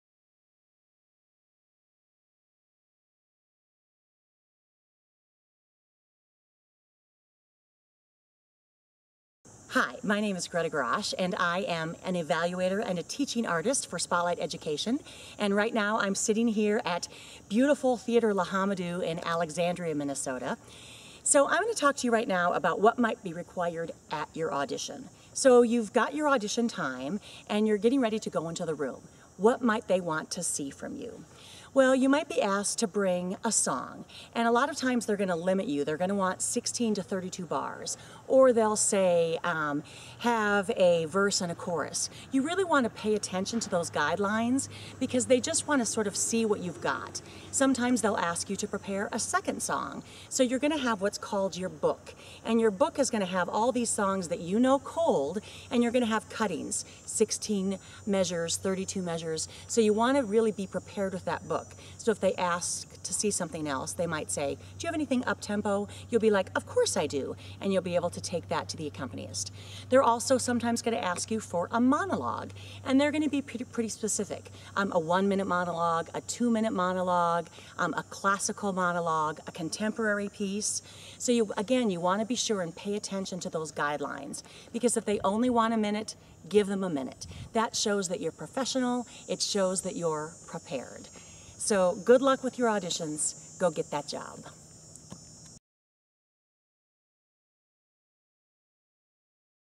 Auditions: What You Need
Twisted Plays is proud to have helped produce this new video series from our friends at Theatre L'Homme Dieu.  Here's the first of the series.  The rest will be located on their channel.  Check it out!